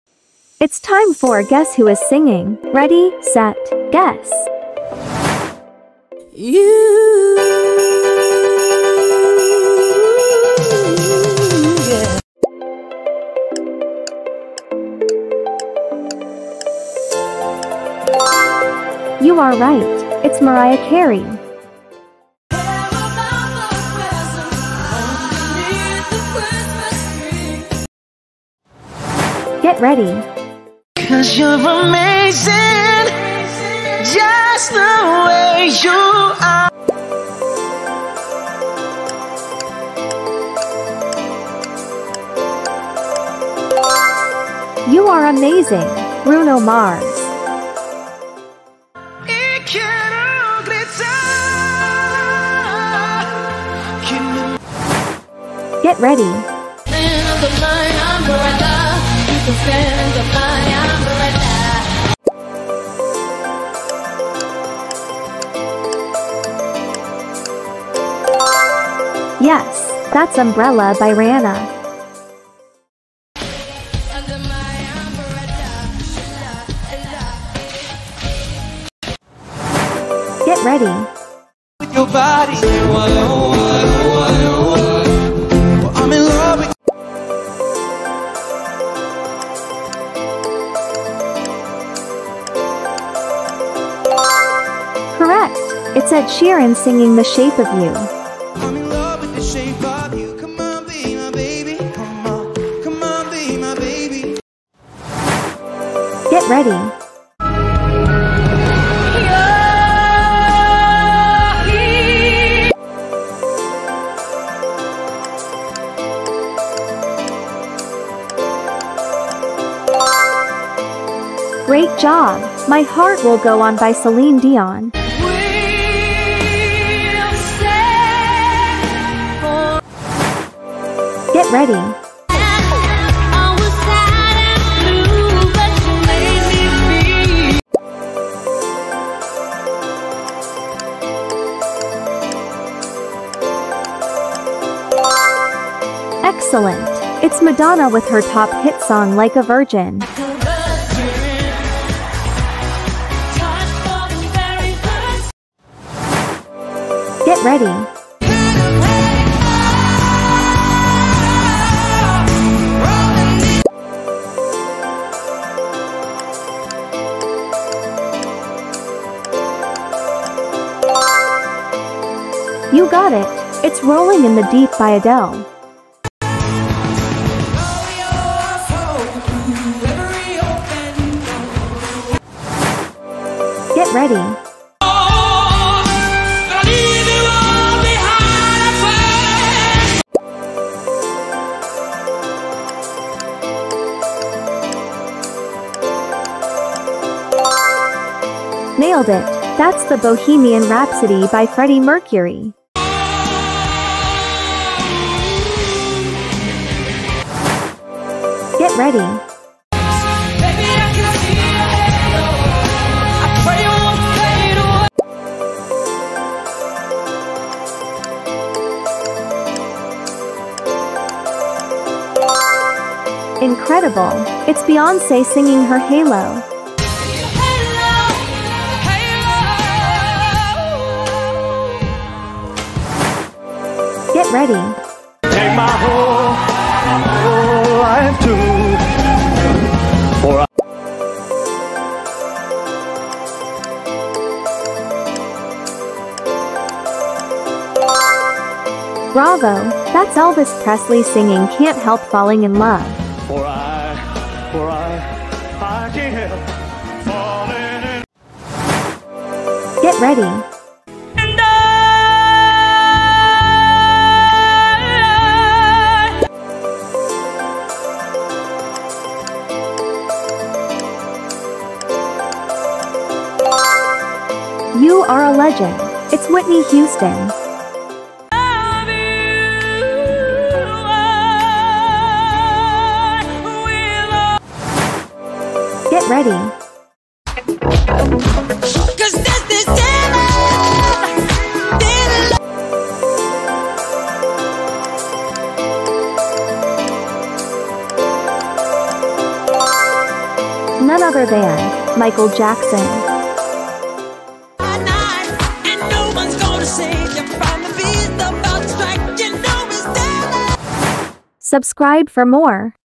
Guess Who Is Singing 🎤🎶 | Beyonce, Rihanna, Adele, Michael Jackson, Ed Sheeran, Bruno Mars, Mariah
Guess Who's Singing 🎤🎶 | Beyonce, Rihanna, Adele, Michael Jackson, Ed Sheeran, Bruno Mars, Mariah

🎤 Welcome to the ultimate "Guess Who is Singing" challenge! 🎶 Test your music knowledge and see if you can correctly identify the singers behind these amazing performances. From chart-toppers to timeless classics, this game will put your ears to the test!

Listen to each snippet carefully and make your best guess on who the talented vocalist is. Challenge your friends and family to join in on the fun! Comment your answers below and let's see who the true music aficionados are.